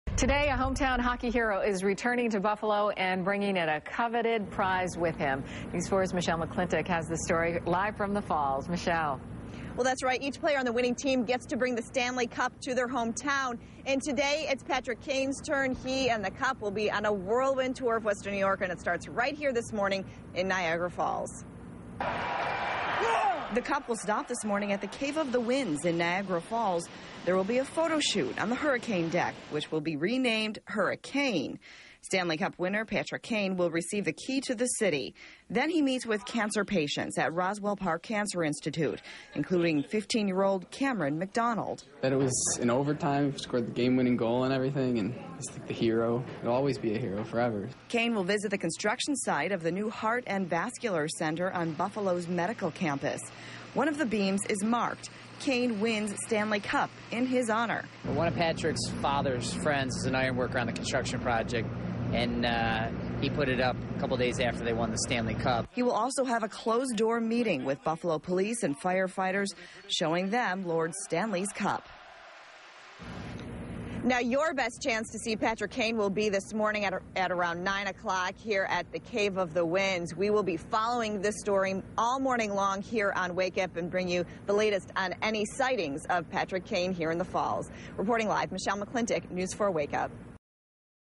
Hometown hockey hero hosts Stanley Cup
Friday, a hometown hockey hero is returning to Buffalo, and bringing a coveted prize with him.